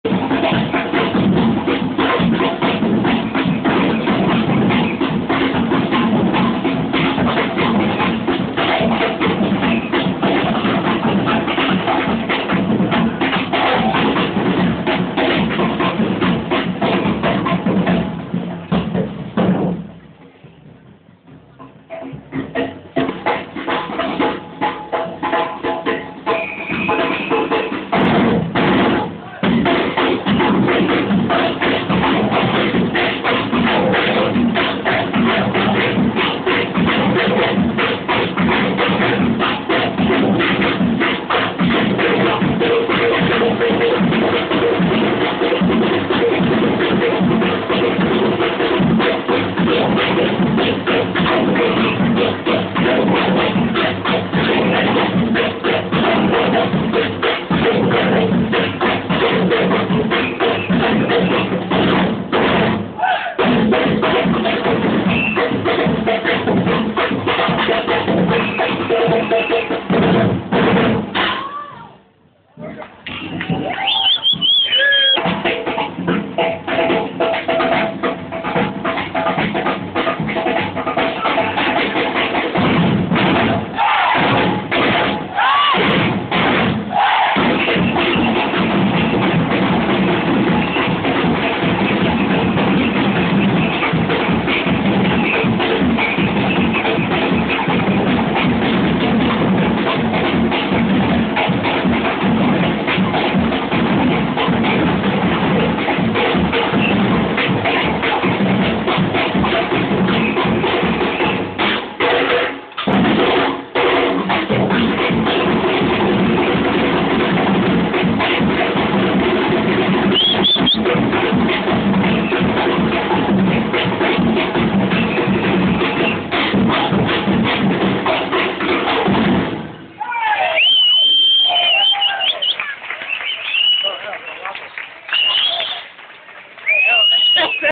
Aste Nagusia 2008 Combe Capelle
Paseando por el casco Viejo nos encontramos con este grupo de percusión y al final de la actuación acabamos comprandoles un cd.